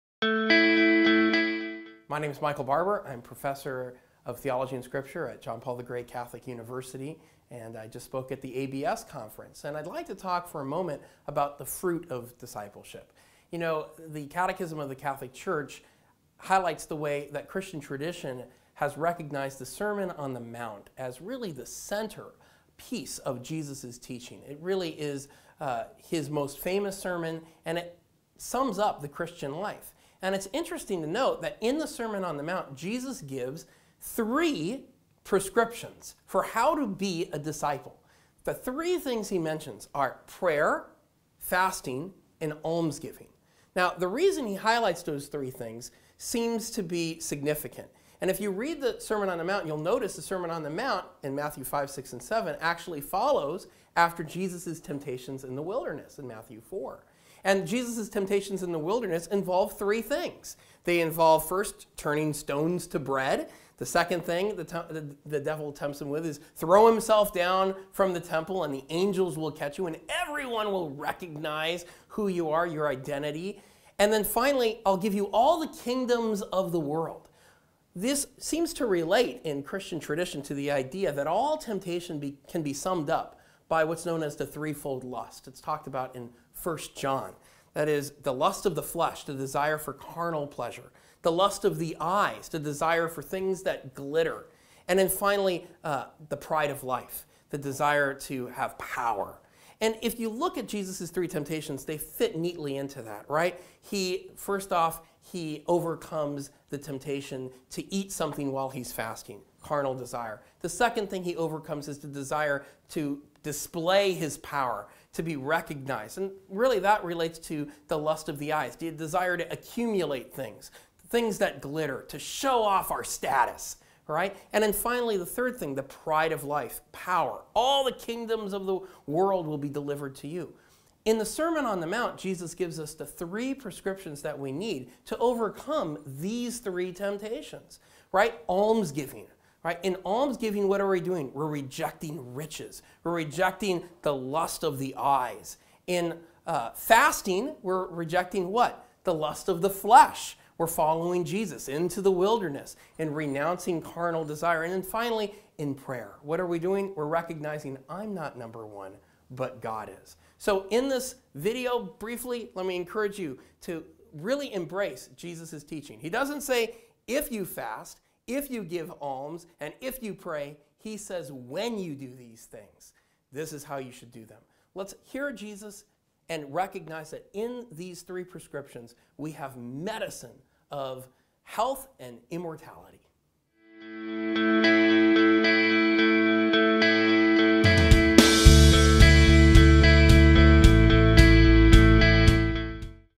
BeADisciple Day 19 (adult track) - Dr. Michael Barber
"The Catechism of the Catholic Church highlights...the Sermon on the Mount as the center piece of Jesus' teaching."

In today's video, Dr. Michael Barber identifies the pitfalls of the spiritual life and the prescriptions for overcoming these temptations.

How can you better embrace the prescriptions of prayer, fasting, and almsgiving in your daily life?